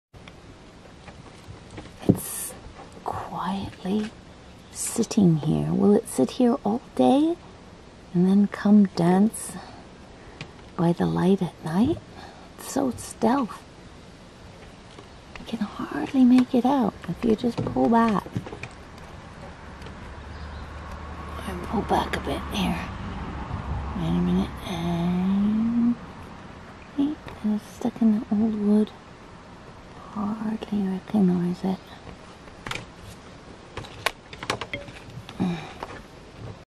Moth Camouflage
Moth camouflage that you can hardly detect during the Day.